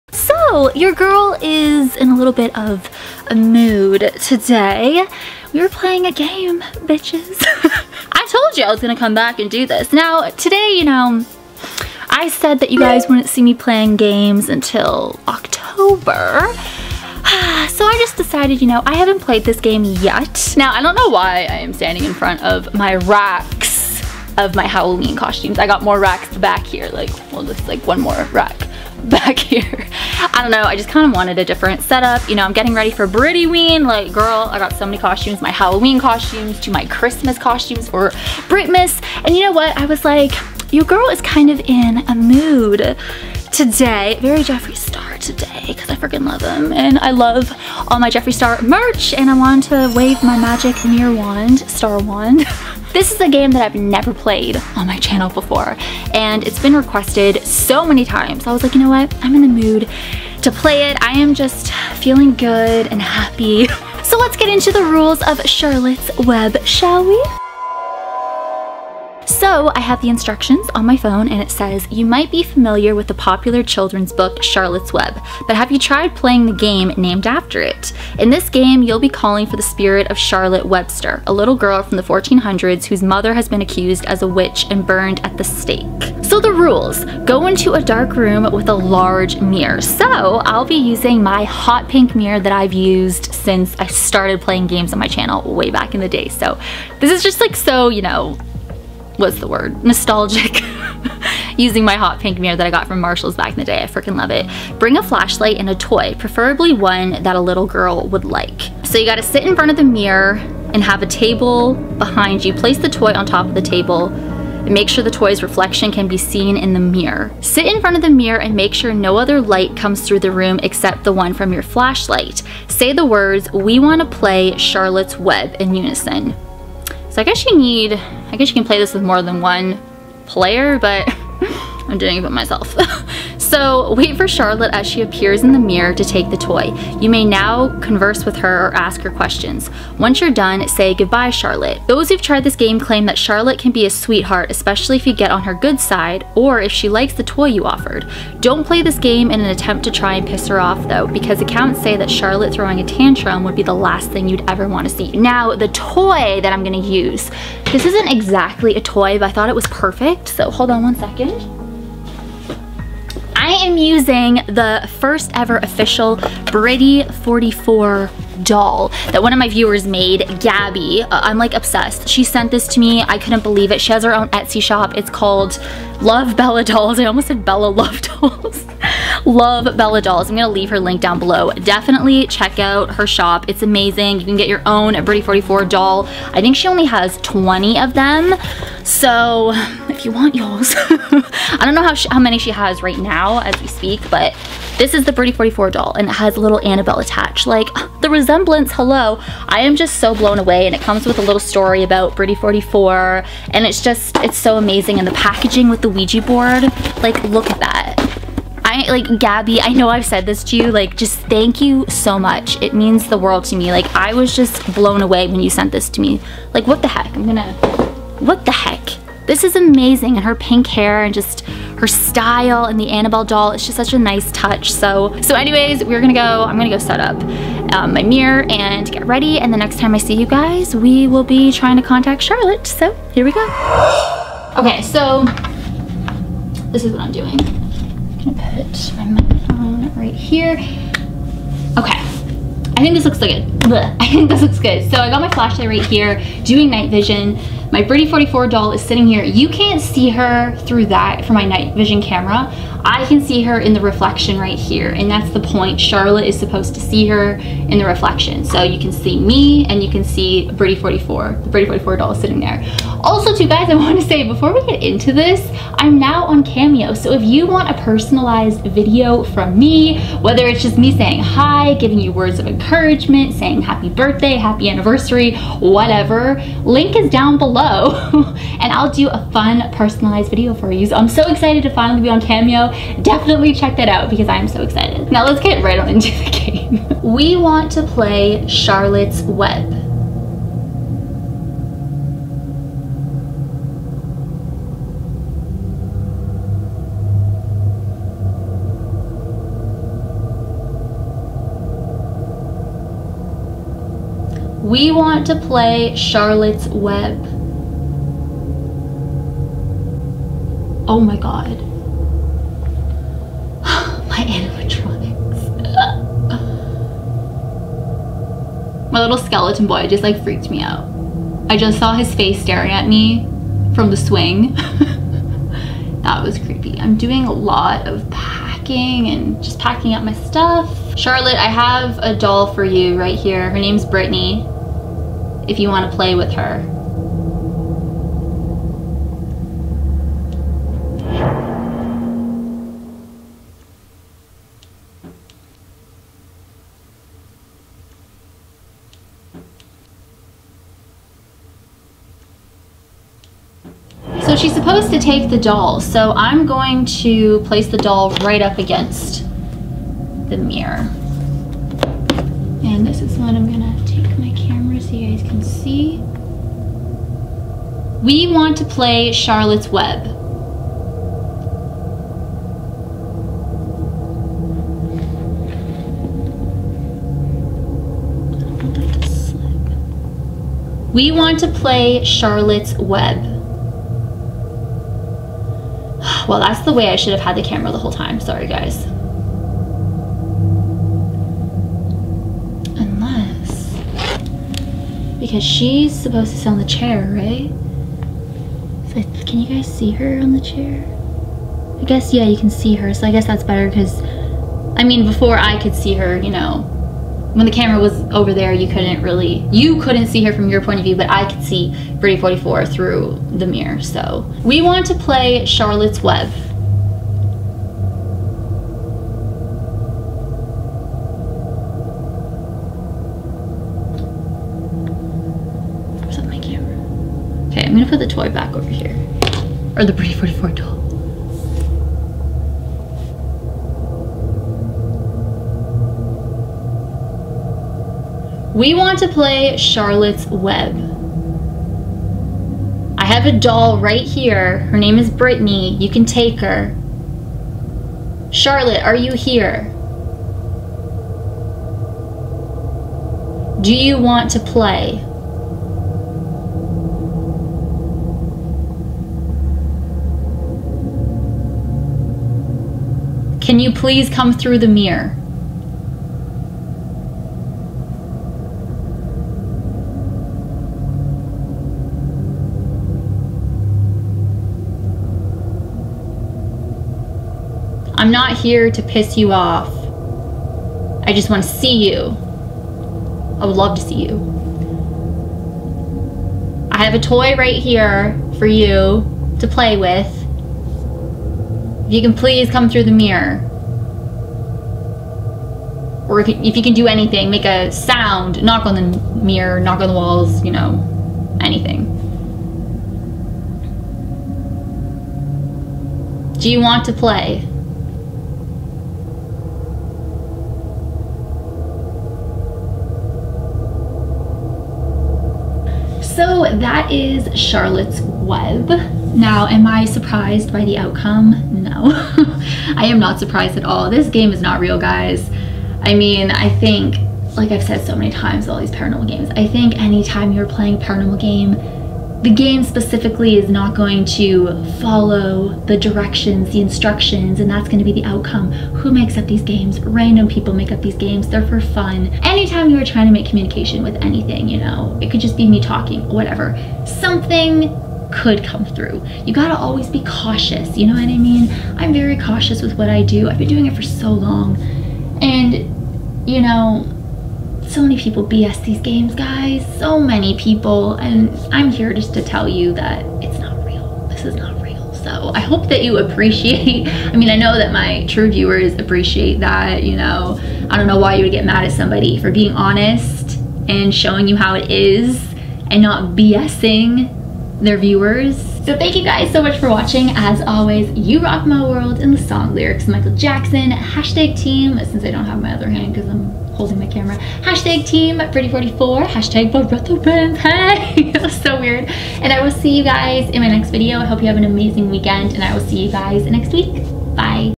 GHOST GIRL in Mirror.. Scary Paranormal Game
Today we are playing the scary paranormal game ghost game known as Charlotte's Web!  This has been a highly requested creepypasta ritual from you guys!  This scary mirror game involves summoning a ghost girl named Charlotte through the mirror by using a creepy doll or toy, even a haunted doll or haunted toy if you want!

I'm on Cameo! Get a personalized video from me!